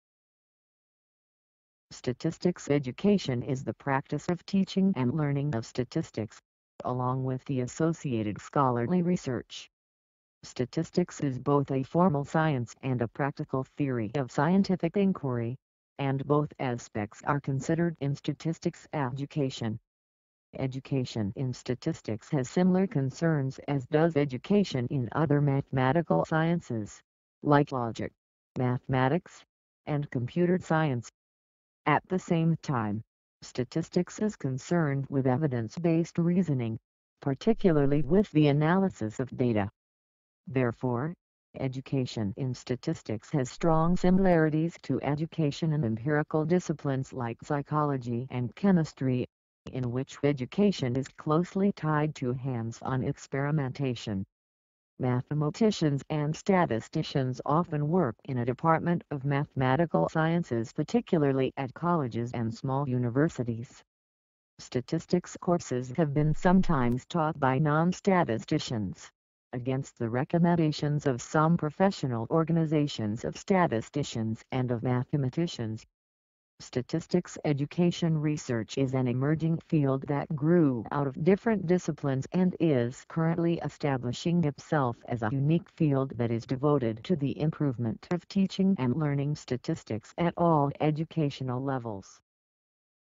What Is STATISTICS EDUCATION? STATISTICS EDUCATION Definition & Meaning
What is STATISTICS EDUCATION, What does STATISTICS EDUCATION mean, STATISTICS EDUCATION meaning, STATISTICS EDUCATION definition, STATISTICS EDUCATION explanation

Statistics education is the practice of teaching and learning of statistics, along with the associated scholarly research.

Statistics is both a formal science and a practical theory of scientific inquiry, and both aspects are considered in statistics education. Education in statistics has similar concerns as does education in other mathematical sciences, like logic, mathematics, and computer science. At the same time, statistics is concerned with evidence-based reasoning, particularly with the analysis of data. Therefore, education in statistics has strong similarities to education in empirical disciplines like psychology and chemistry, in which education is closely tied to "hands-on" experimentation.

Mathematicians and statisticians often work in a department of mathematical sciences (particularly at colleges and small universities). Statistics courses have been sometimes taught by non-statisticians, against the recommendations of some professional organizations of statisticians and of mathematicians.

Statistics education research is an emerging field that grew out of different disciplines and is currently establishing itself as a unique field that is devoted to the improvement of teaching and learning statistics at all educational levels.
Source: Wikipedia.org